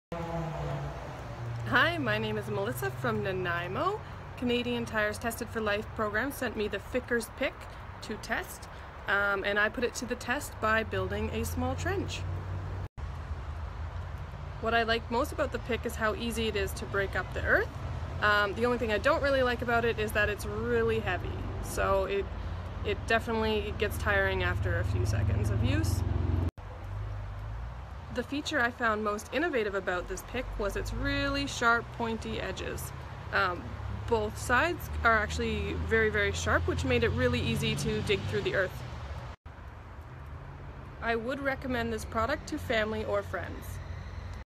My Product Review: Fiskars Steel Pick
Fiskars Pick is ideal for breaking up rocky surfaces such as concrete or hardened earth. Mattock end can be used like a hoe when digging through soil. Ergonomically contoured handle fits naturally in the hand. Bio-dynamically engineered texture offers a secure grip.